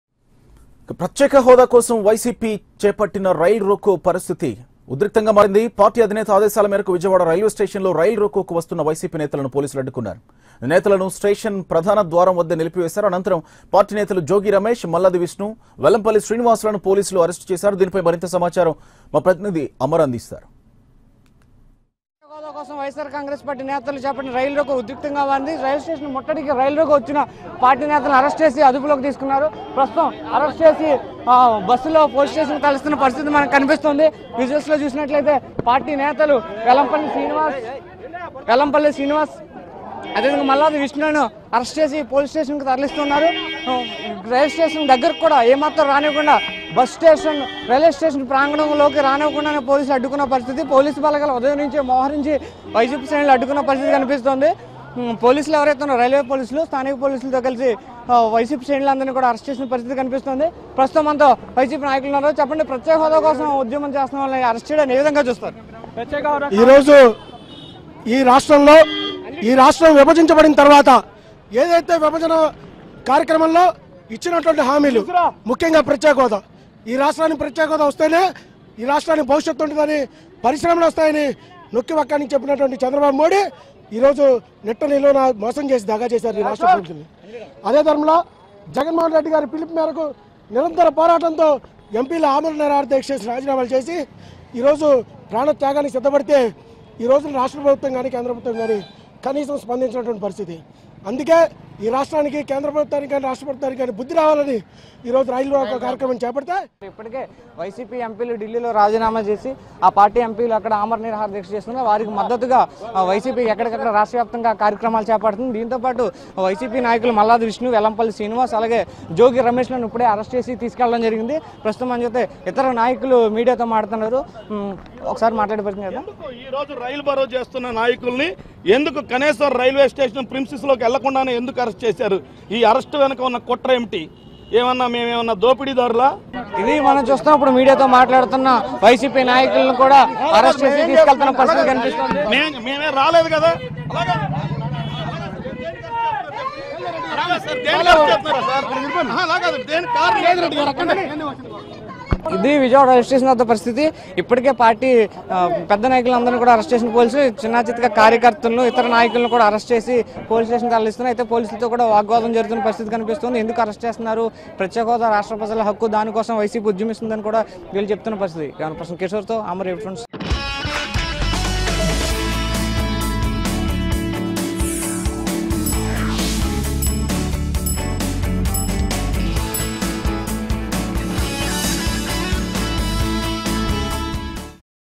Police Held YSRCP Leaders Against Rail Roko In Vijayawada | AP24x7
Police held YSRCP leaders against rail roko in Vijayawada who were protesting for AP special status and slam centre for not fulfilling the bifurcation guarantees.